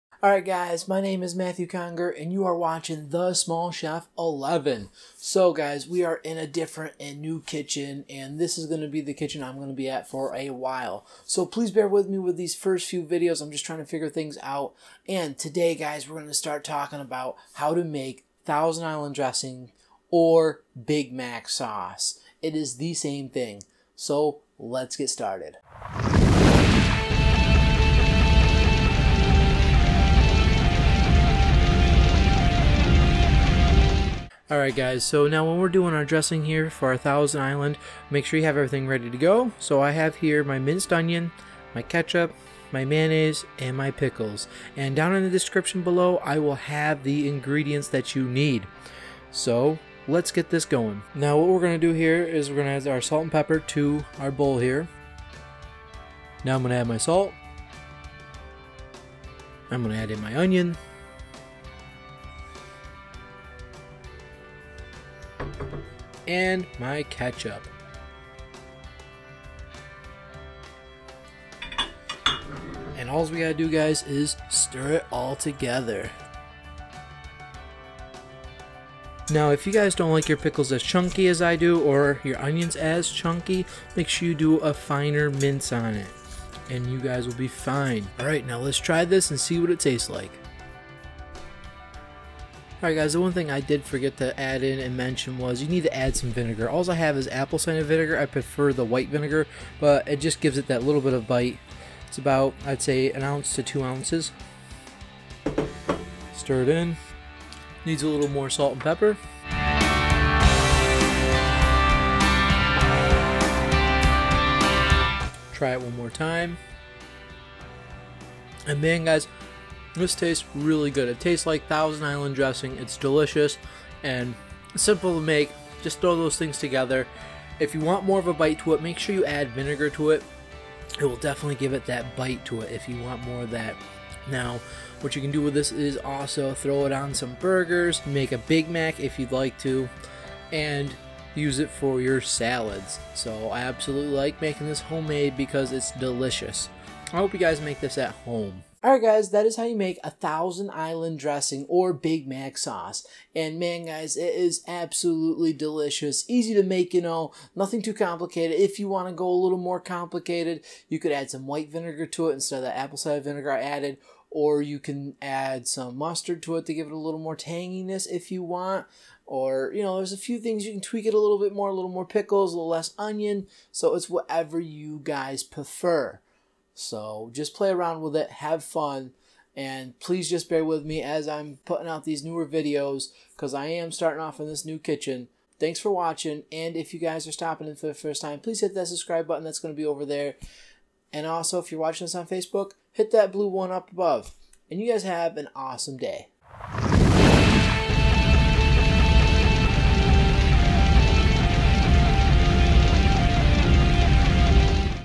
Big Mac Sauce (Simple)
how to make Big Mac Sauce (Simple)

Ingredients

1-3 Dill pickles
1-2 oz white vinegar
2 cups mayonnaise
1 small Yellow onion minced
1/2 cup ketchup
TT salt
TT pepper

TT= To Taste

Always a big thanks to burt beebe